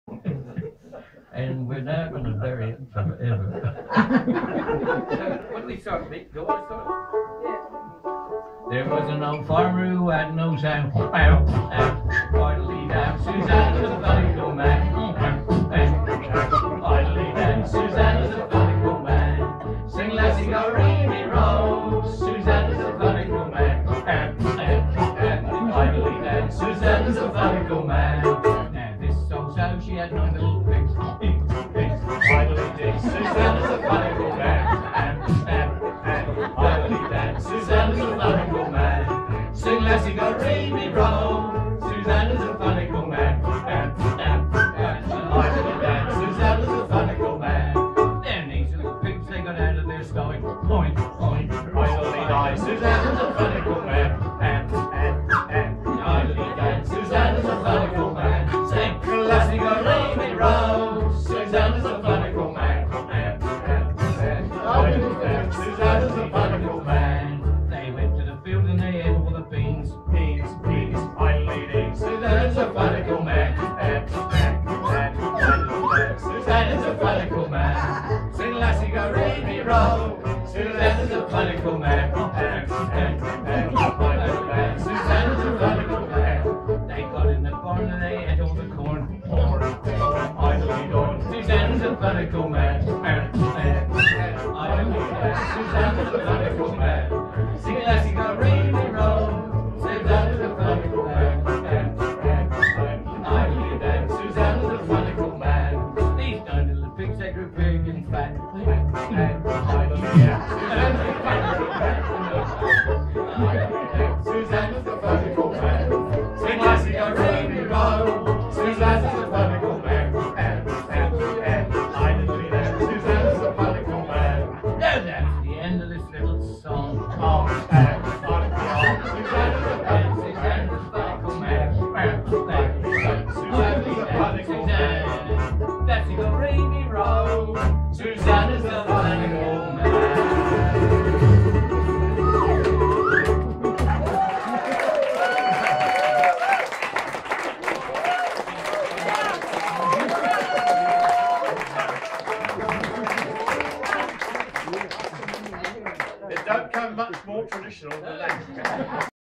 Sourbelly String Band
10 June 2015 performing in The Bunker for the Auckland Bluegrass and Old Time Country Music Club.